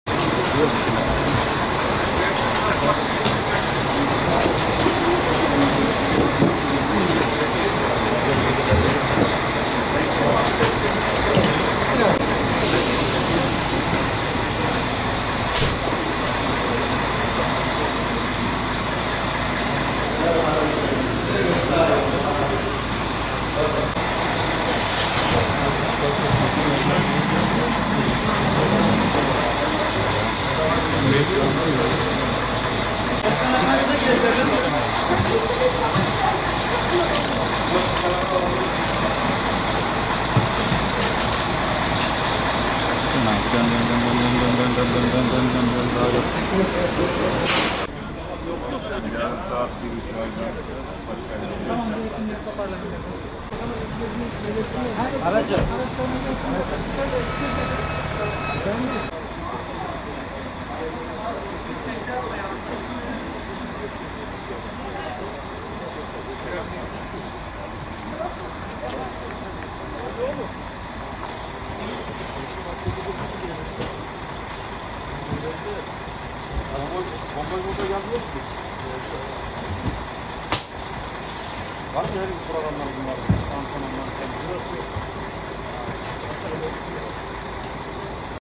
Fenerbahçe Trabzon’da! | Ersun Yanal'la SON KEZ...
Ziraat Türkiye Kupası yarı final ilk maçında yarın Trabzonspor’a konuk olacak Fenerbahçe, Trabzon’a geldi.
Ziraat Türkiye Kupası yarı final ilk maçında 3 Mart Salı günü Trabzonspor’a konuk olacak Fenerbahçe, Süper Lig’de Fraport TAV Antalyaspor ile deplasmanda oynayacağı maçın ardından Antalya’da kupa maçı hazırlıklarını tamamlayarak Trabzon’a geldi. Saat 17.40 sıralarında Trabzon’a inen sarı-lacivertli kafile daha sonra geniş güvenlik önlemleri altında  konaklayacağı otele geçerek maç saatini beklemeye başladı.